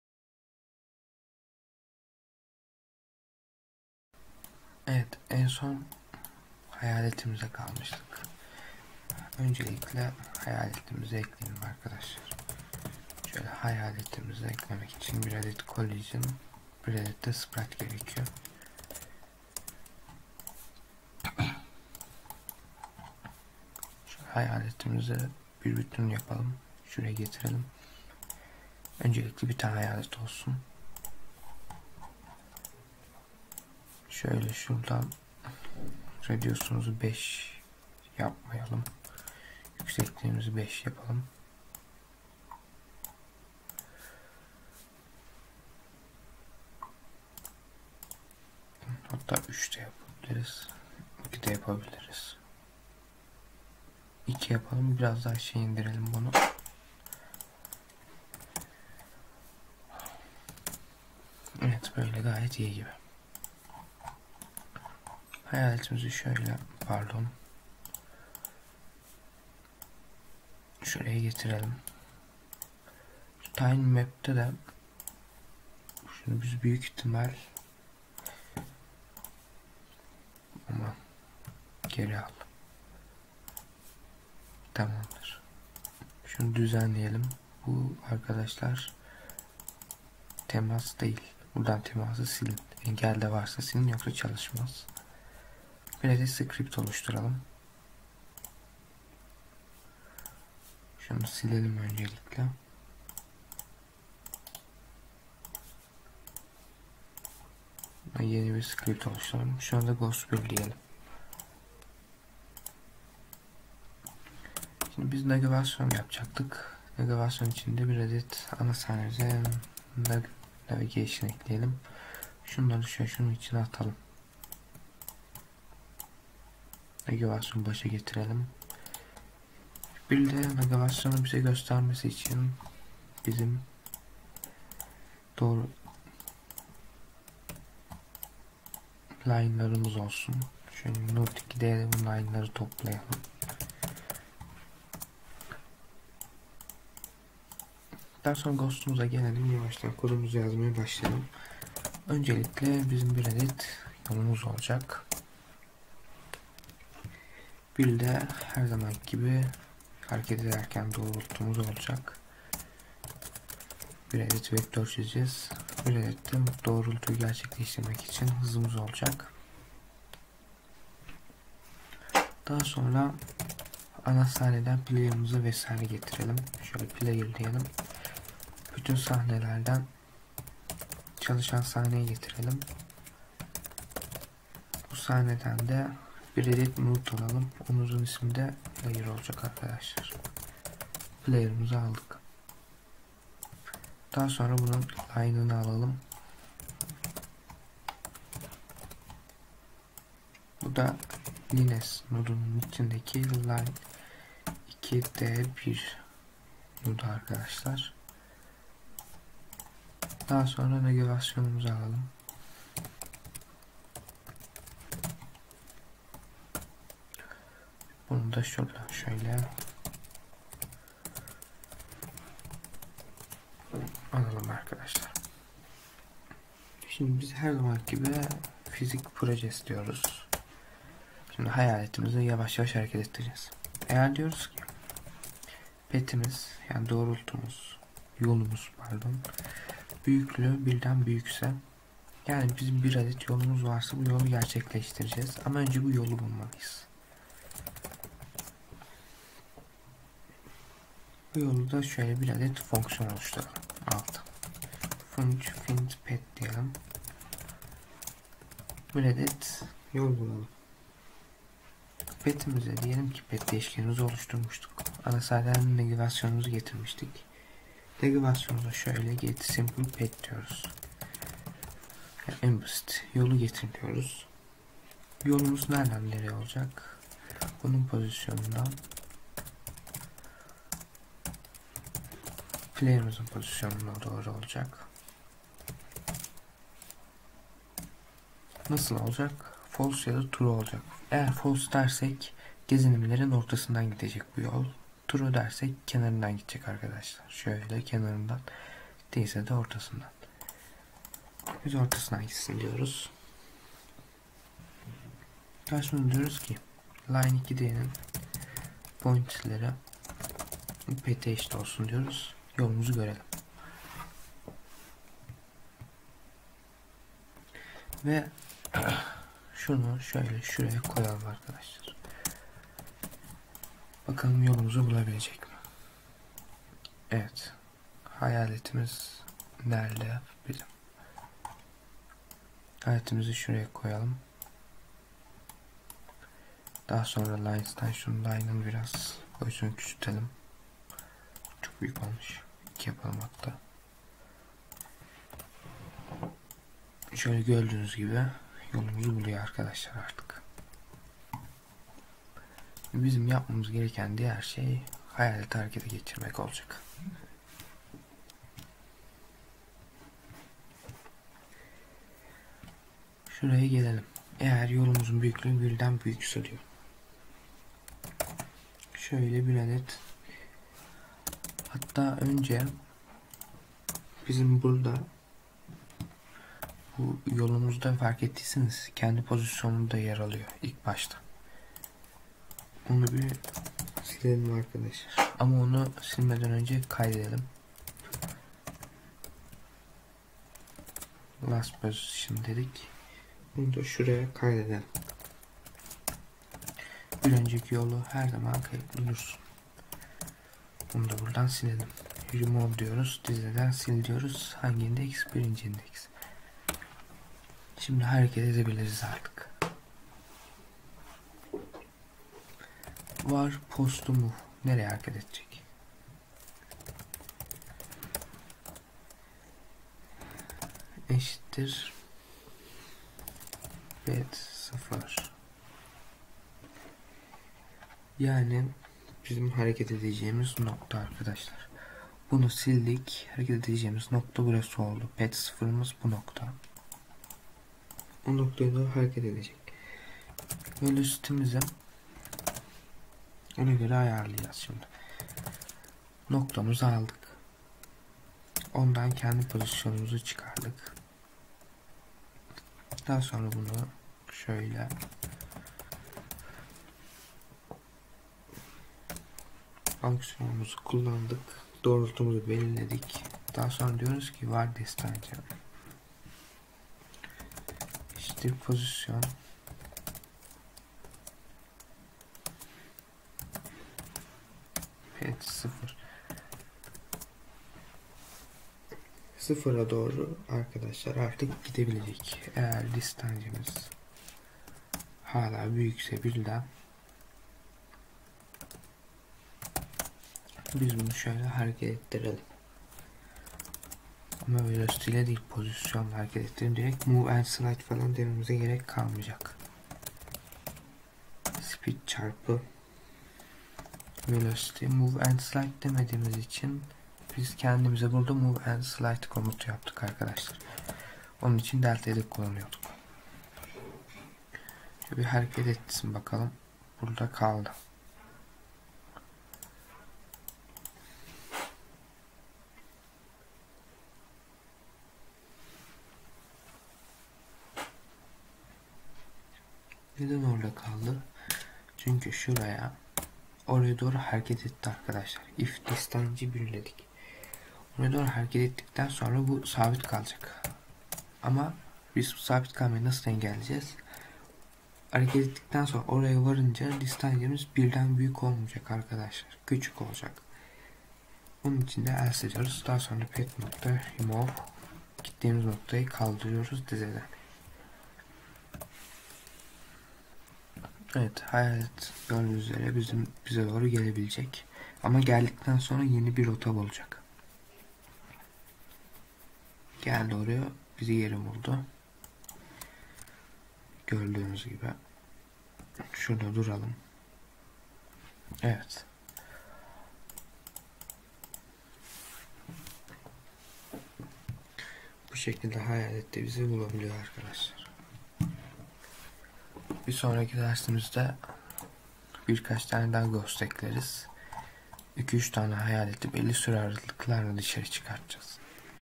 Godot Engine Ders 8 PACMAN Oyunu Yapımı Detaylı ve Basit Türkçe Hayalet NPC Yapay Zeka Navigation2D
Derslerin devamı gelecektir. Oyunun bitmiş hali web sayfamızda mevcuttur.